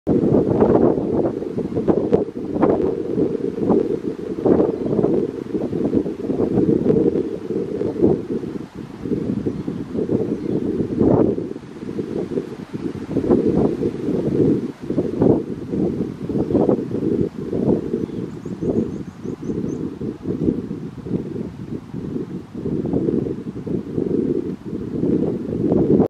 Slobeenden in het Dwingelderveld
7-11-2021
Slobeenden in het Holtveen in het Dwingelderveld.